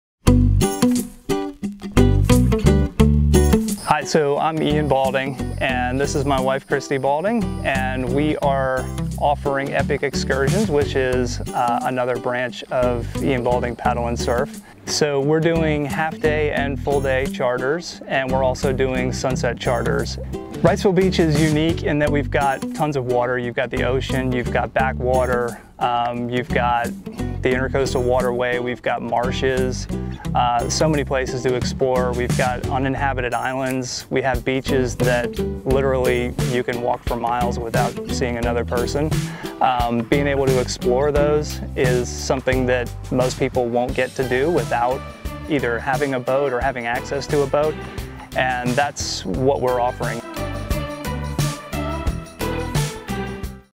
Epic Excursions in Wrightsville Beach, NC
Embark on a half- or full-day charter or sunset cruise with Epic Excursions. For a different kind of adventure add paddleboards or surf boards to your tour. Get out on the water to explore our waterways, marshes and uninhabited islands in Wrightsville Beach, NC.